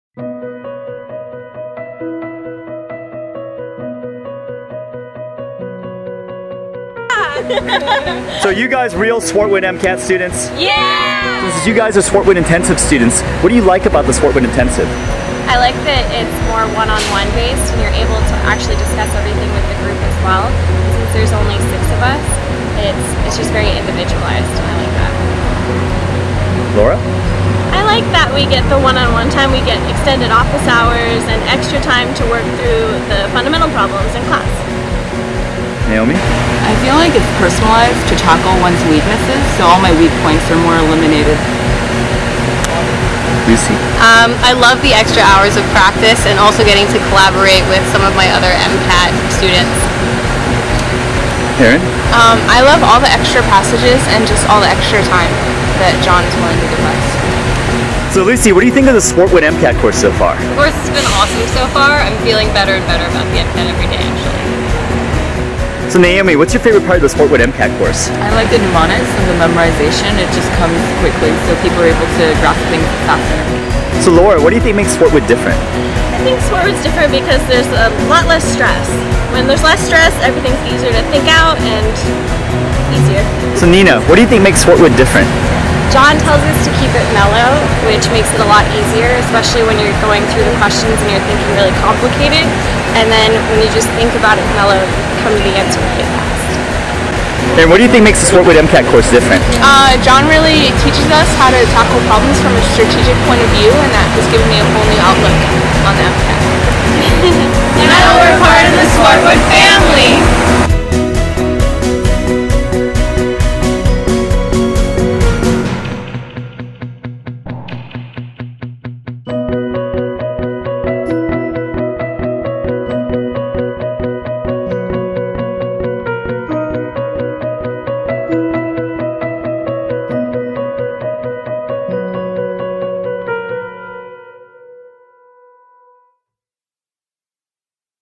SW Intensive Video
A brief testimonial from some SWARTWOOD Intensive students about the SW Intensive MCAT Course.  The SW Intensive meets with the larger SW Only but also has small group sessions with John.